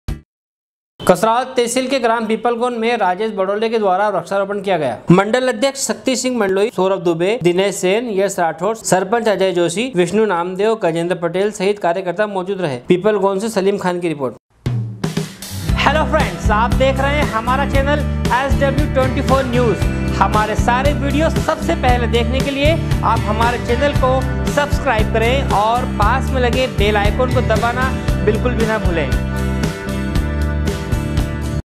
SW 24 News Madhyapradesh
कसरावद तहसील के ग्राम पीपलगोन में राजेश जी बड़ोले, के द्वारा वृक्षारोपण किया गया। पीपलगोन से सलीम खान की रिपोर्ट